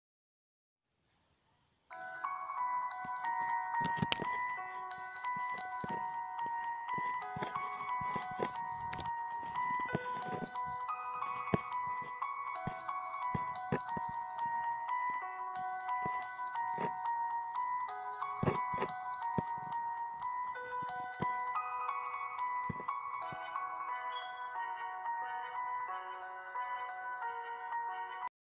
Bath time! | Undertale animation
Asriel takes a bath but falls out of the tub and ya know.